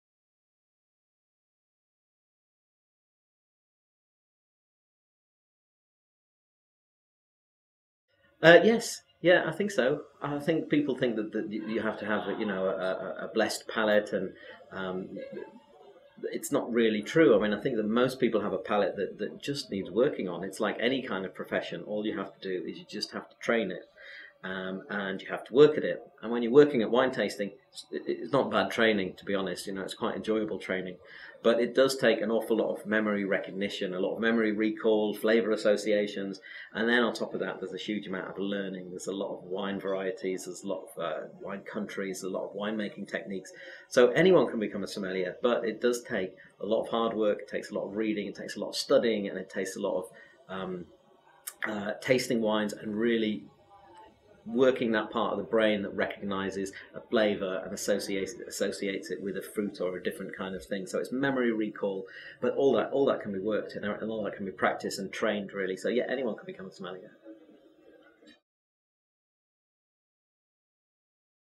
Ronan Sayburn answers: "Can anyone become a Sommelier?"
We sat down with Ronan Sayburn and asked him all about being one of the UK's leading Master Sommeliers.

In this clip, we asked Ronan: "Can anyone become a Sommelier?"